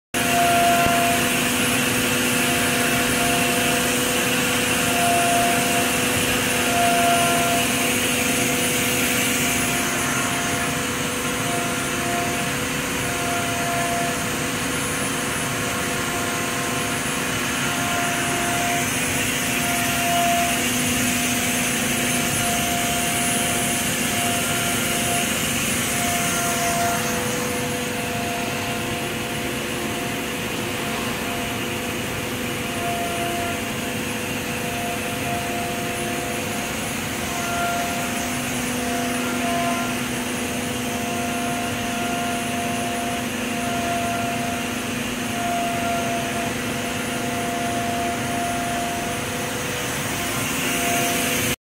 JETiger High Pressure Coolant DrivenJet Spindle
Demonstration of the JETiger with RPM monitor display on a Haas VF-3ss More Speed + More Torque.